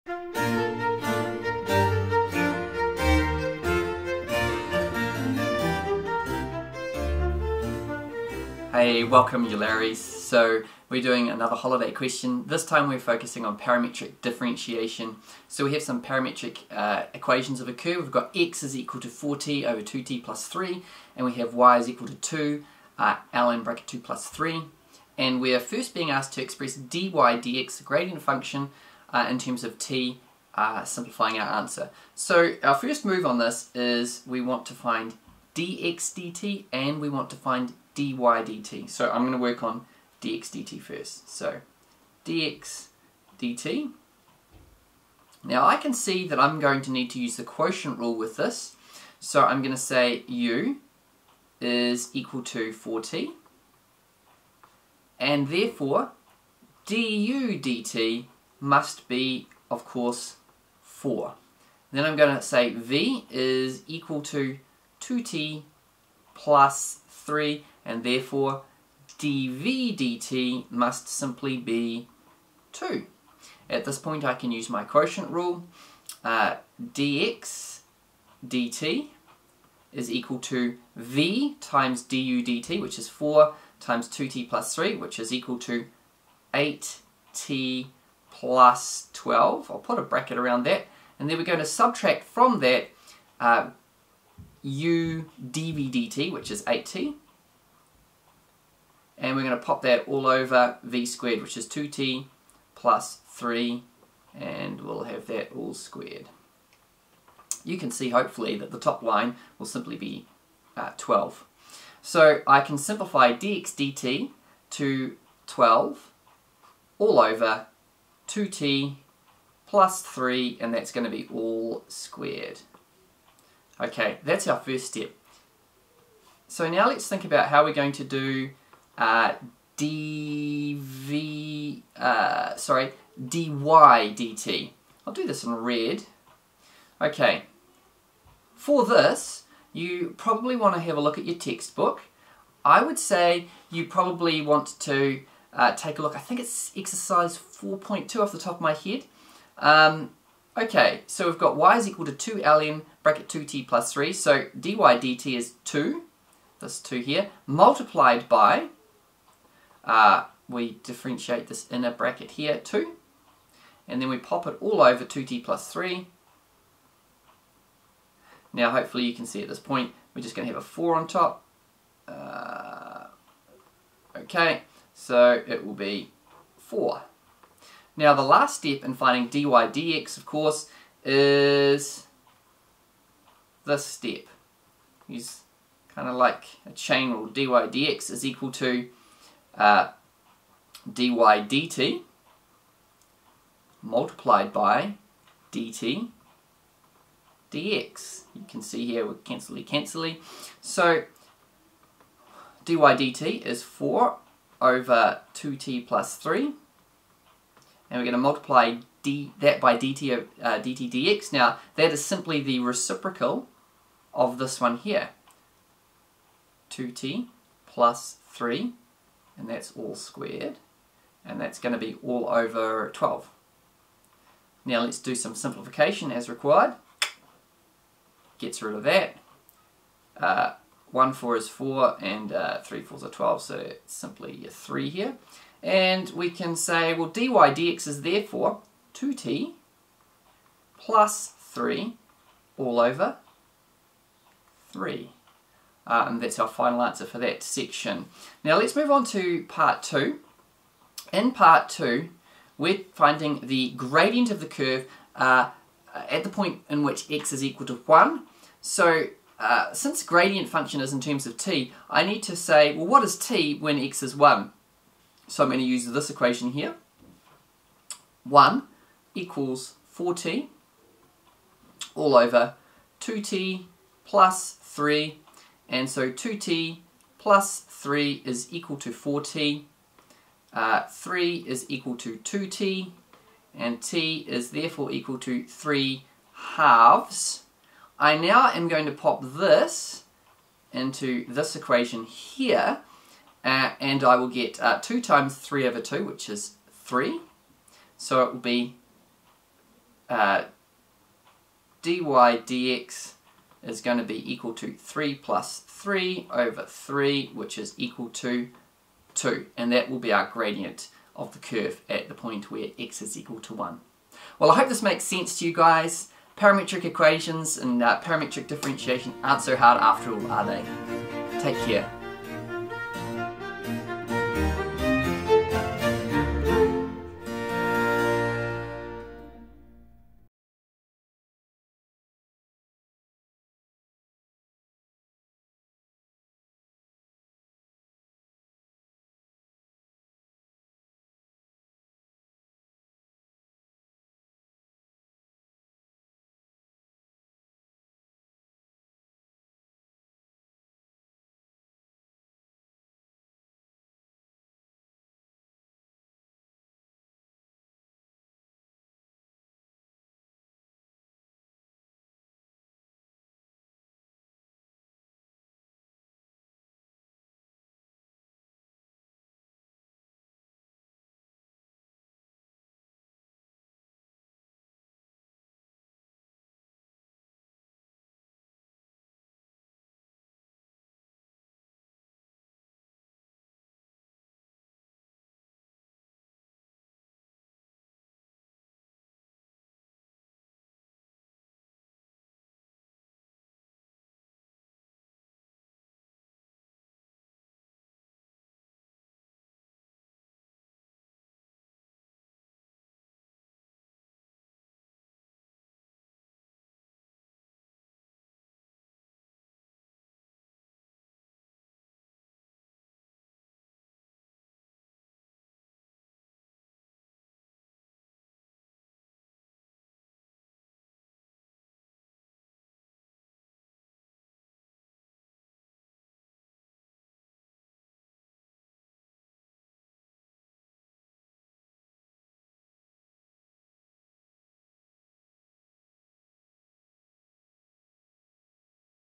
Pure Maths 3 - Holiday Practice Question 3 - Parametric Differentiation
A worked example of a past Cambridge examination question for Pure Maths 3 on parametric equations and differentiation.